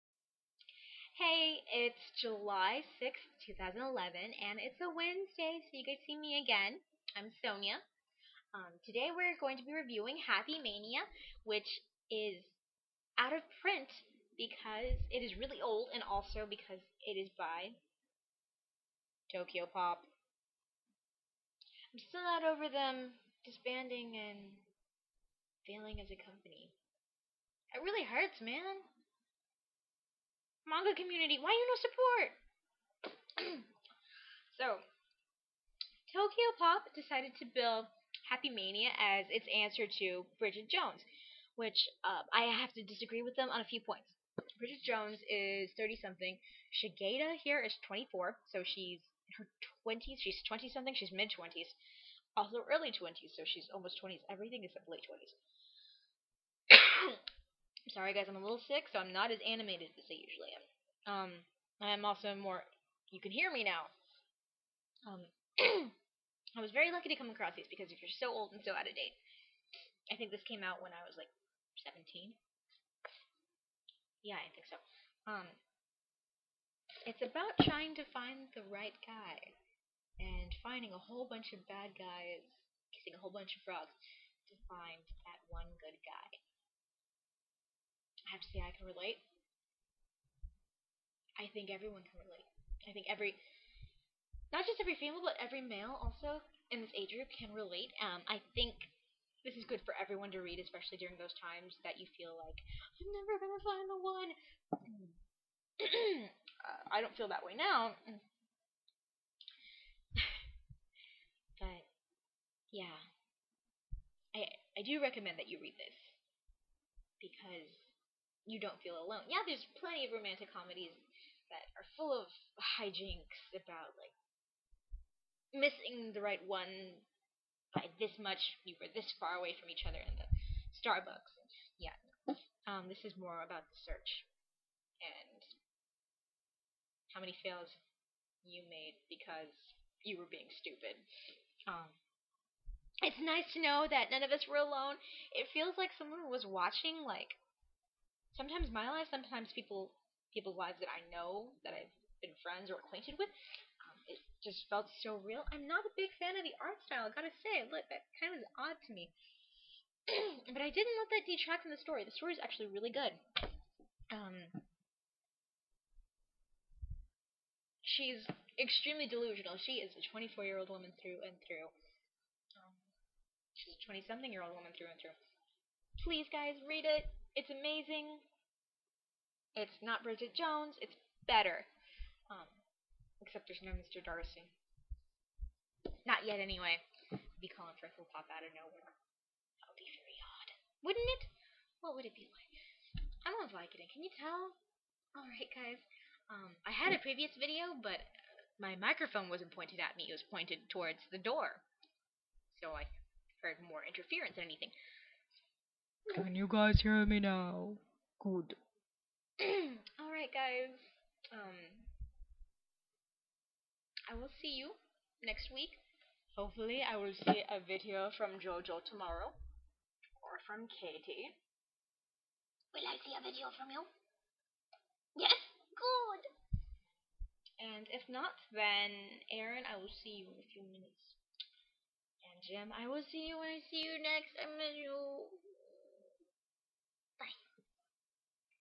A very happy Sonya reviews Happy Mania
SONYA TOOK VICODIN AND REVIEWED A MANGA. OMG!